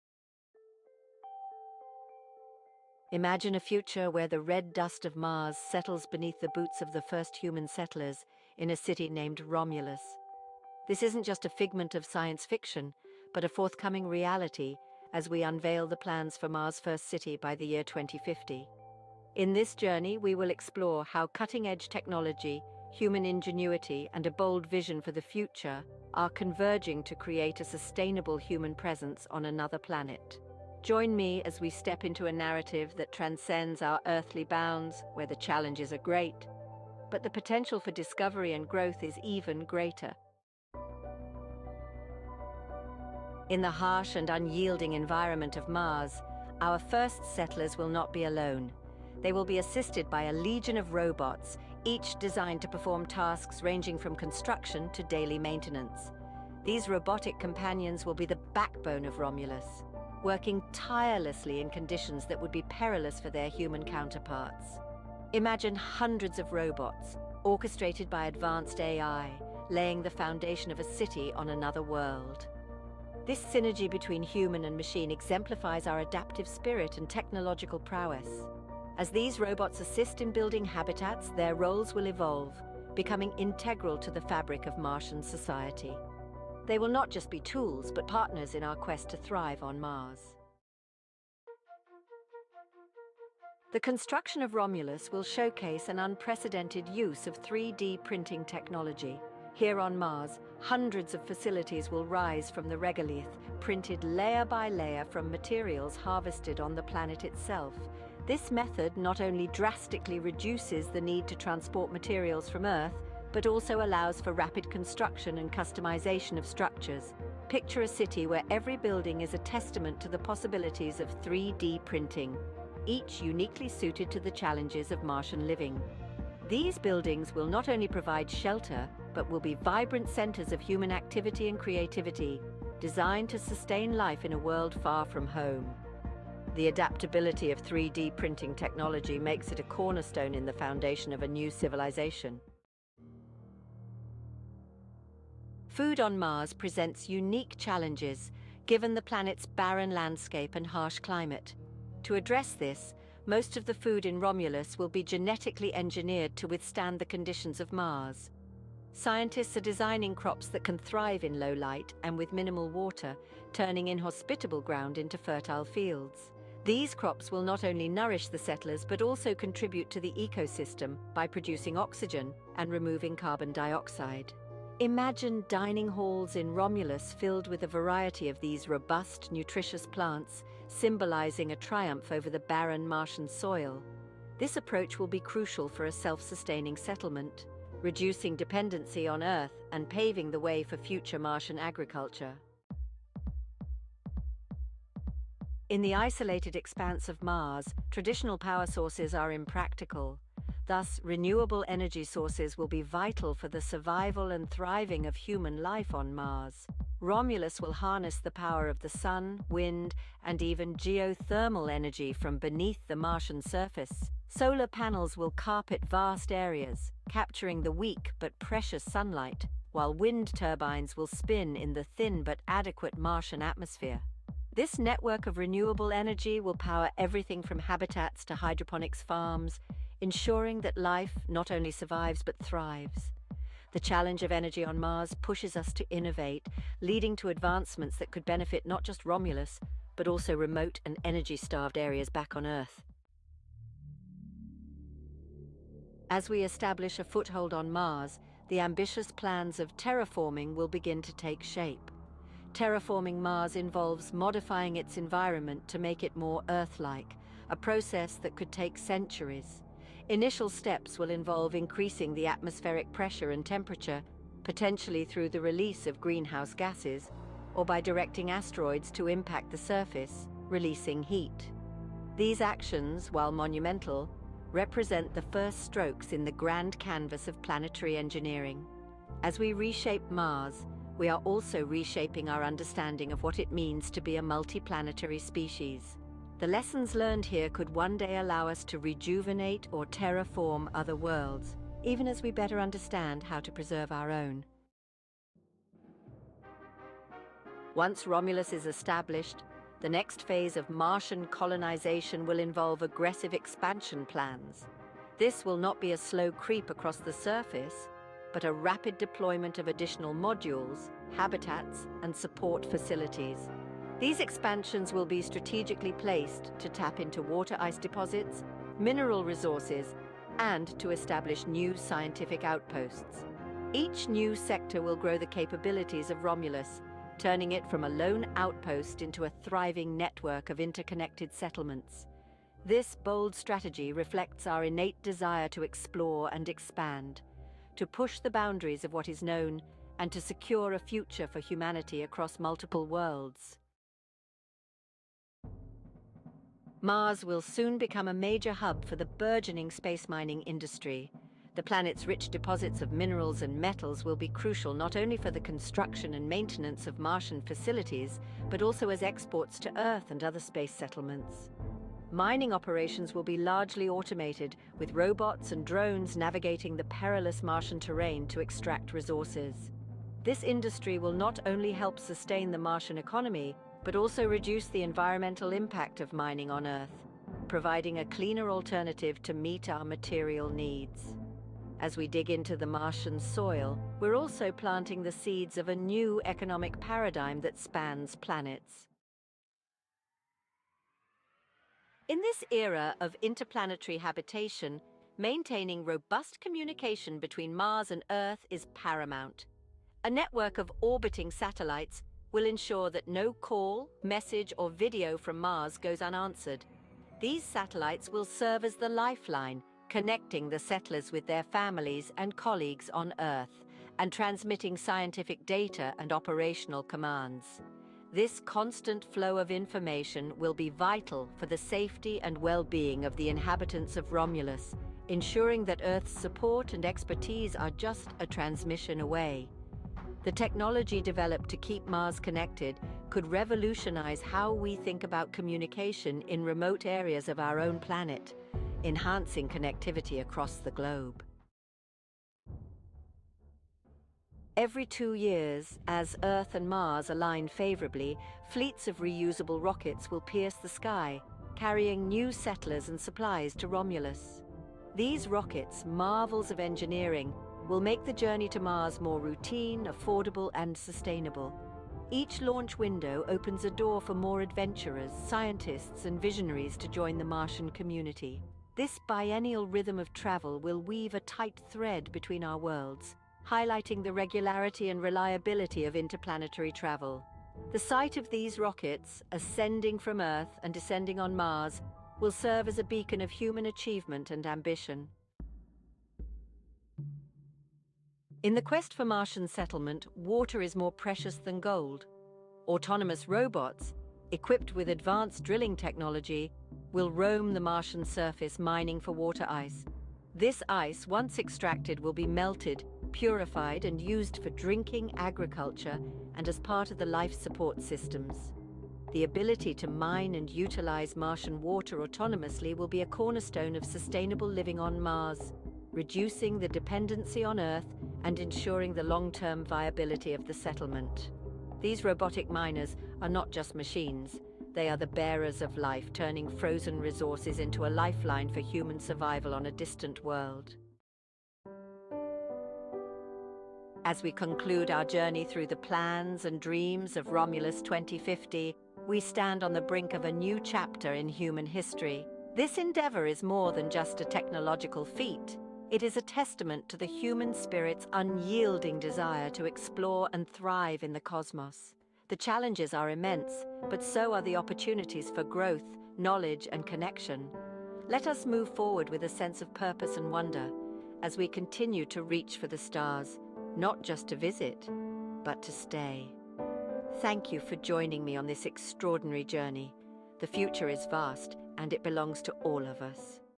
Romulus 2050: Mars' First City
Explore the bold vision for humanity's first city on Mars, set to be established by 2050. Delve into cutting-edge technology and human ingenuity as we uncover the convergence of robotics, AI, 3D printing, renewable energy, and more to create a sustainable Martian civilization.

From autonomous robots handling construction and maintenance tasks to genetically engineered crops thriving in Martian conditions, this video showcases the incredible advancements that will pave the way for our species' expansion into the solar system. Witness the evolution of Martian infrastructure, from the rapid deployment of habitats to the strategic utilization of local resources.

Discover how renewable energy, satellite communications, and reusable rockets will power and connect this new frontier. Dive into the challenges and opportunities of terraforming Mars, and how the lessons learned here could shape the future of planetary engineering.

Join us as we unveil the remarkable plans to establish Romulus, the first city on Mars, and witness the convergence of human ingenuity and technological prowess that will propel us towards a bold and sustainable future in space.

OUTLINE:
00:00:00
Romulus 2050

00:00:40
Robotic Companions of Mars

00:01:30
Building a Future with 3D Printing

00:02:22
Genetically-Engineered Sustenance

00:03:12
Powering a New World

00:04:02
Reshaping Mars

00:04:54
Expansion Beyond the Horizon

00:05:43
Mining the Red Frontier

00:06:28
Mars to Earth

00:07:21
The New Trailblazers

00:08:05
The Martian Gold

00:08:50
A New Chapter in Human History